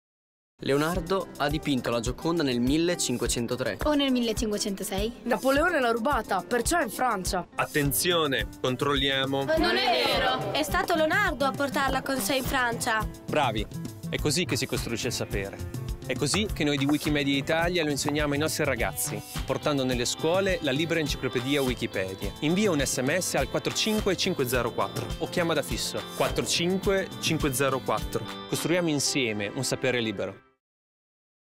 SMS solidale Wikimedia Italia
Con un SMS o una chiamata da fisso al 45504 aiuterai Wikimedia Italia a promuovere i nostri corsi su Wikipedia nelle scuole italiane e a insegnare alle giovani generazioni l'importanza di un sapere libero, condiviso e facilmente accessibile.